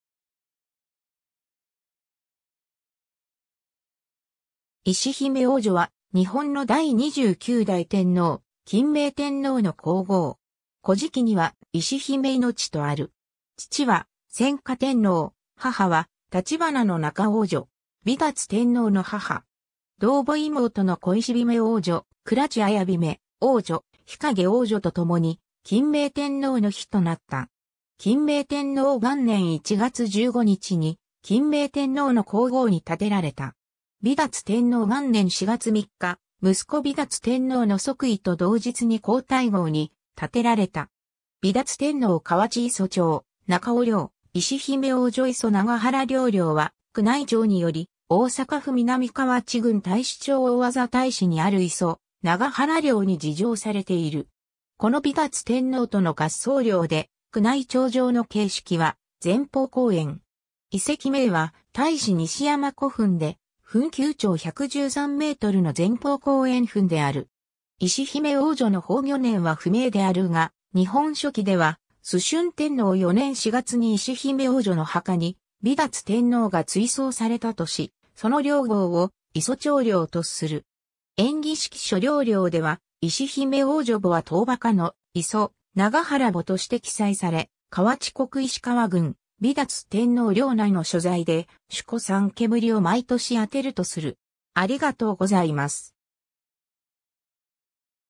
石姫皇女
日本の神_(人物神_歌人)
石姫皇女（いしひめのひめみこ/いしひめのおうじょ、生没年不詳）は、日本の第29代天皇・欽明天皇の皇后。
『古事記』には石比売命（いしひめのみこと）とある。
 父は宣化天皇、母は橘仲皇女（仁賢天皇の皇女）。
敏達天皇の母。
 同母妹の小石姫皇女（記のみ）・倉稚綾姫皇女・日影皇女（以上紀のみ）と共に欽明天皇の妃となった。
欽明天皇元年1月15日（540年2月8日）に欽明天皇の皇后に立てられた。
敏達天皇元年4月3日（572年4月30日）、息子敏達天皇の即位と同日に皇太后に立てられた。
 敏達天皇 河内磯長中尾陵・ 石姫皇女 磯長原陵 （大阪府南河内郡太子町） 陵（みささぎ）は、宮内庁により大阪府南河内郡太子町大字太子にある磯長原陵（しながのはらのみささぎ）に治定されている。
子の敏達天皇との合葬陵で、宮内庁上の形式は前方後円。
遺跡名は「太子西山古墳」で、墳丘長113メートルの前方後円墳である。
 石姫皇女の崩御年は不明であるが、『日本書紀』では崇峻天皇4年（591年?）4月に石姫皇女の墓に敏達天皇が追葬されたとし、その陵号を「磯長陵」とする。
『延喜式』諸陵寮では石姫皇女墓は遠墓の「磯長原墓」として記載され、河内国石川郡敏達天皇陵内の所在で、守戸3烟を毎年あてるとする。